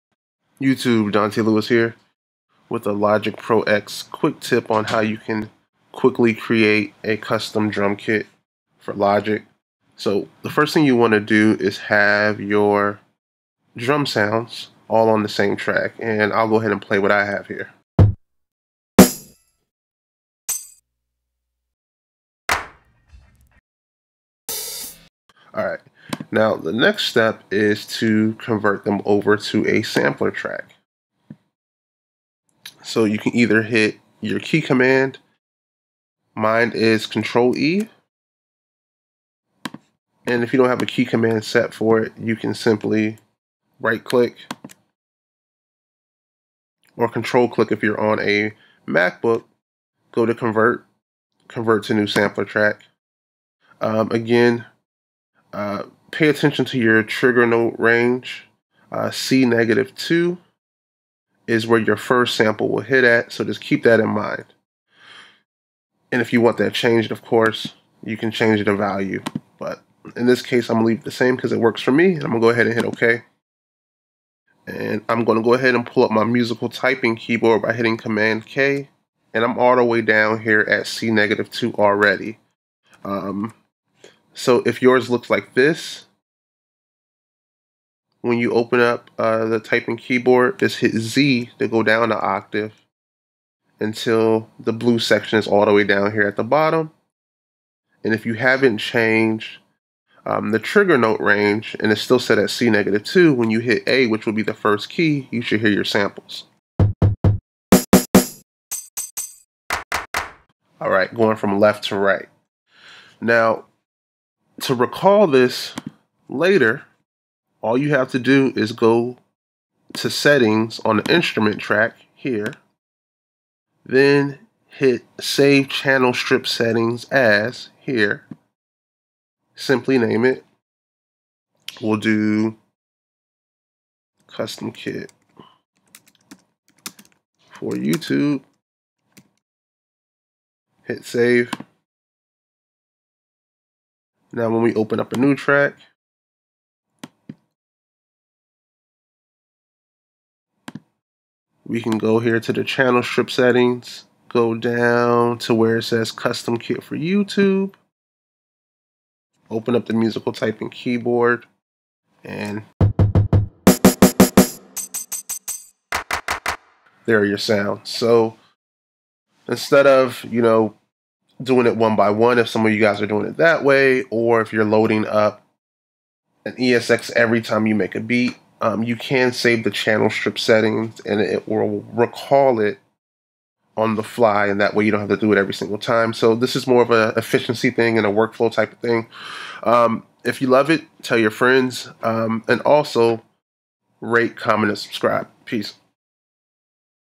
Logic Pro X - Making Custom Drumkits
In this video I will show you how to create a custom drumkit for LPX and how to recall the kits you create for future sessions.